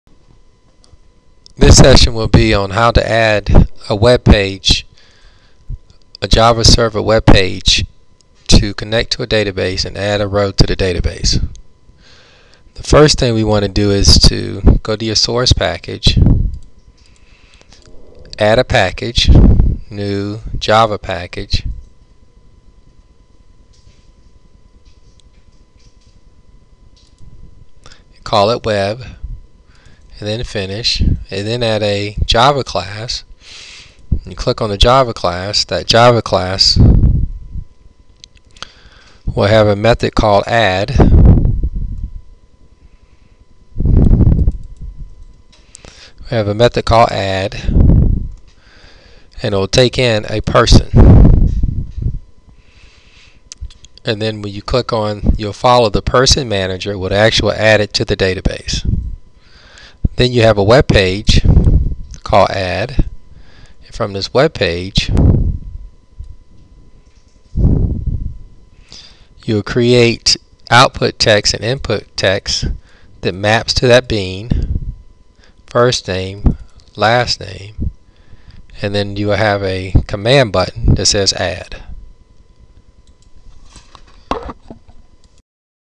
Add Java Server Faces page that inserts data to the database
How to add a JSF page that will add data to the database